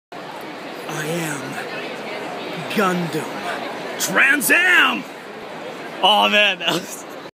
I am.... Gundam
Met brad swaile at fan expo 2013, got him to say his favorite line from Gundam 00 as his character Setsuna F. Seie